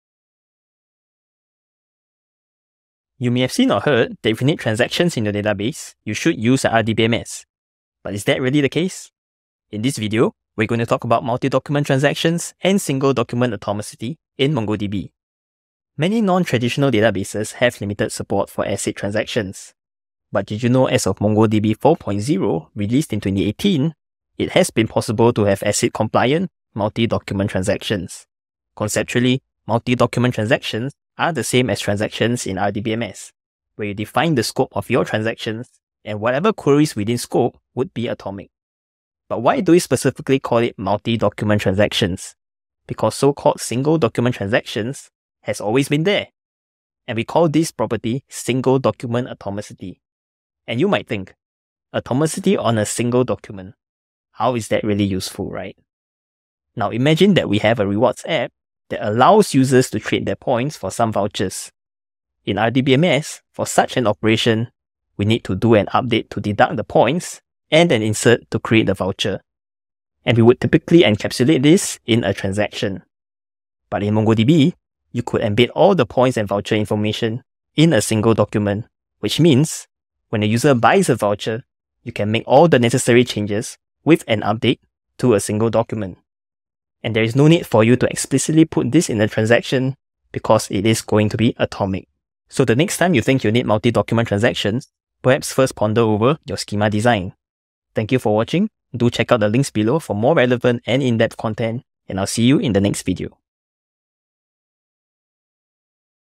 Multi-Document Transaction and Single Document Atomicity
Explore the necessity of multi-document transactions in MongoDB! Learn about MongoDB's approach to transactions and single document atomicity, redefining database integrity and schema design.